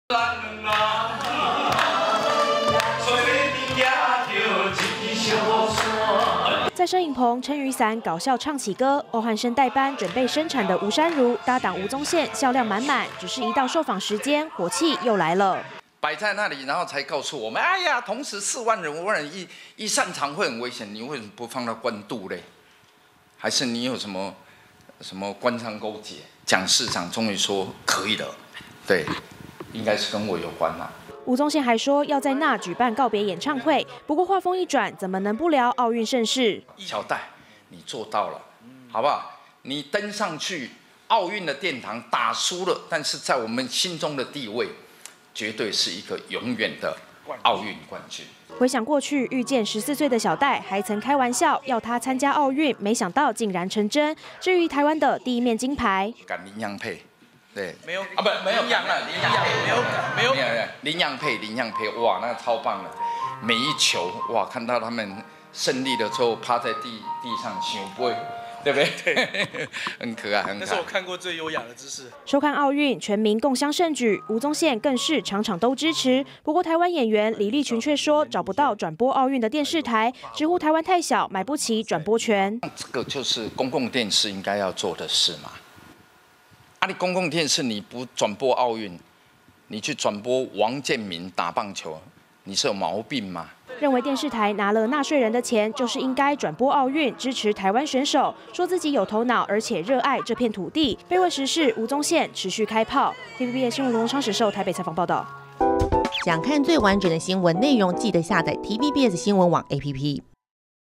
吳宗憲挺奧運「場場都看」! 暖心喊話戴資穎 大讚麟洋配｜TVBS新聞
全面一起瘋奧運，就連藝人也不例外，像是綜藝天王吳宗憲就說，基本上他場場都看，支持中華隊國手。6號下午現身，還暖心喊話戴資穎，「妳永遠是我們心中的奧運冠軍」，也提到這次的羽球男雙麟洋配，直呼他們表現超棒。